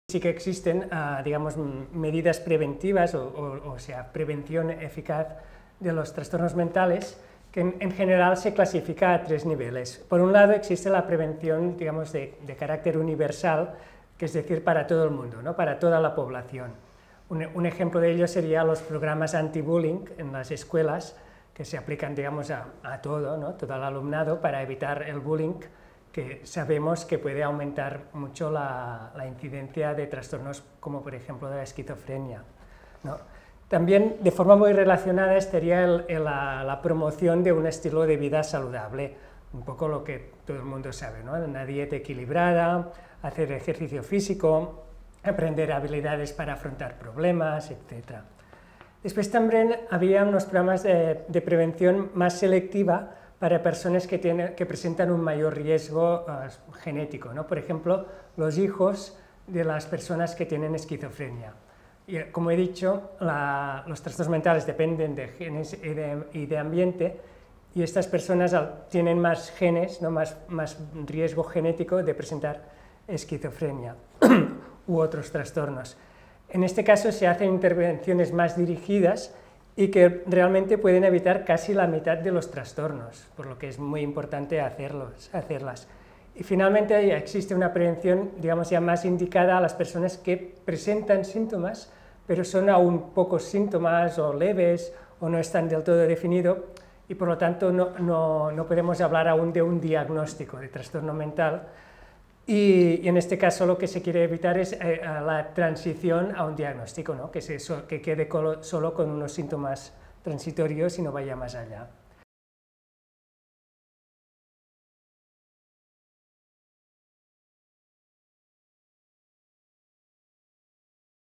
Prevenció de trastornos de salut mental. ¿Es posible eliminarlos? | PortalCLÍNIC
El Dr. Joaquim Raduà, jefe del grupo Imagen de los trastornos relacionados con el estado de ánimo y la ansiedad del IDIBAPS e investigador del CIBERSAM, y la Dra. Gisela Mezquida, neuropsicóloga investigadora del Clínic y del CIBERSAM, responden dudas y preguntas sobre la prevención de trastornos de salud mental.

Más información sobre ansiedad y otros trastornos de salud mental en PortalCLÍNIC